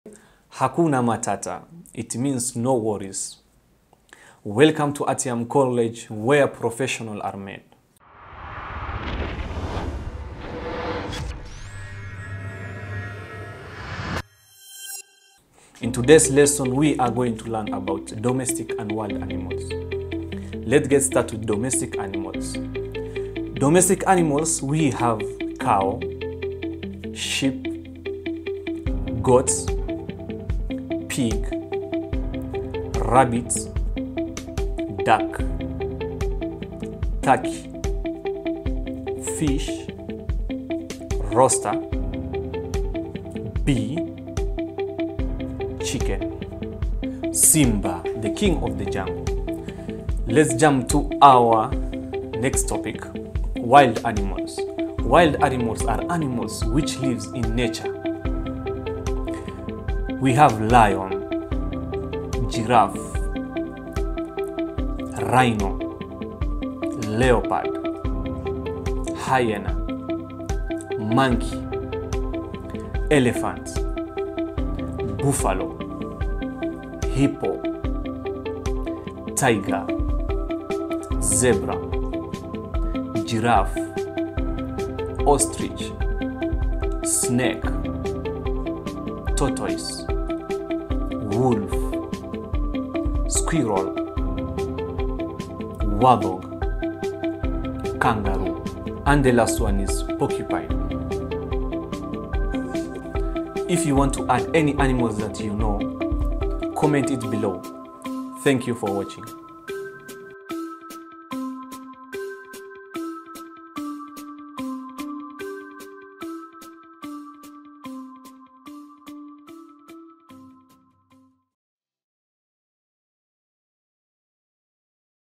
Domestic & Wild animals - English Vocabulary | Learn English
Educational video for English learners to learn new vocabulary and find out about wild and domestic animals